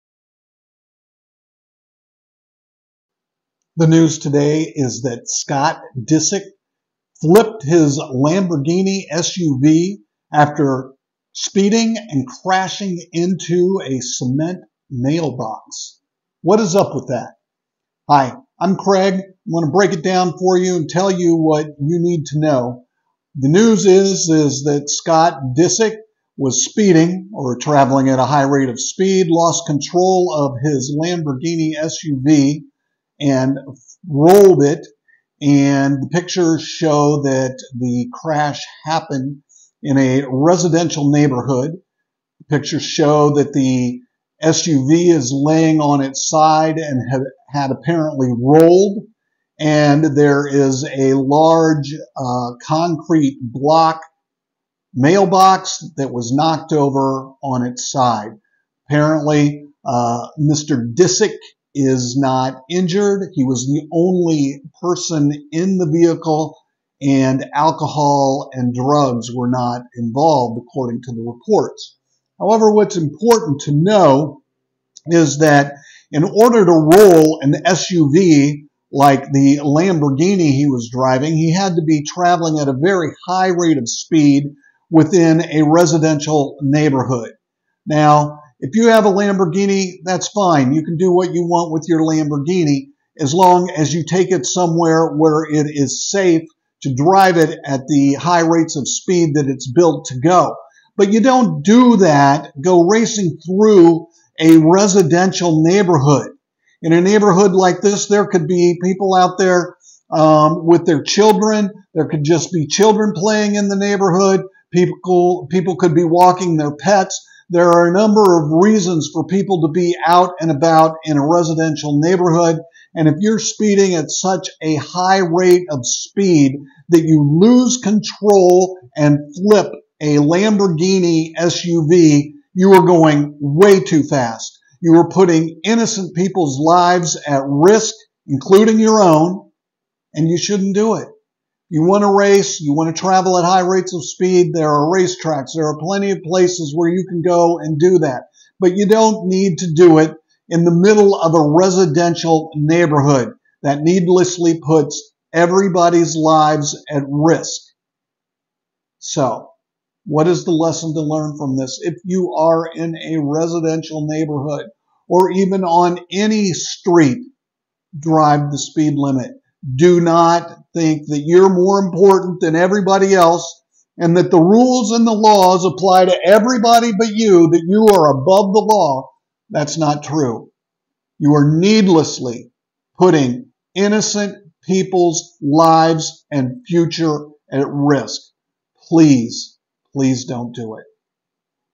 Lawyer Explains Scott Disick Lamborghini Rollover Crash What You Really Need To Know
Craig Murphy of Murphy & Murphy Law Offices discussed the impact of the Scott Disick Lamborghini rollover crash.